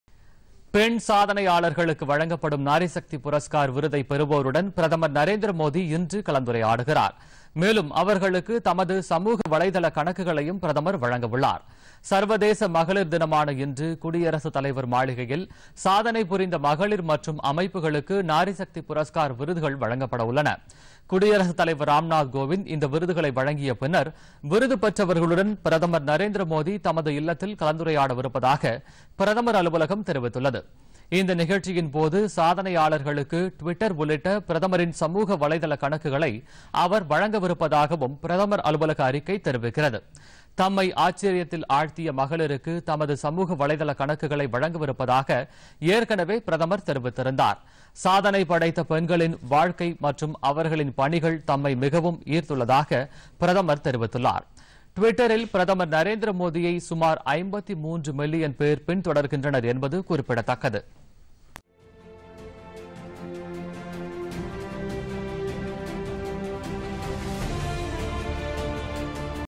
விருது பெறும் பெண்களுடன் பிரதமர் கலந்துரையாடல்
விருது பெறும் சாதனைப் பெண்களுடன் பிரதமர் நரேந்திர மோதி தமது இல்லத்தில் கலந்துரையாடுகிறார் - சமூக வலைதள பக்கங்களையும் மகளிருக்கு வழங்க முடிவு.Visit : TamilNews - Doordarshan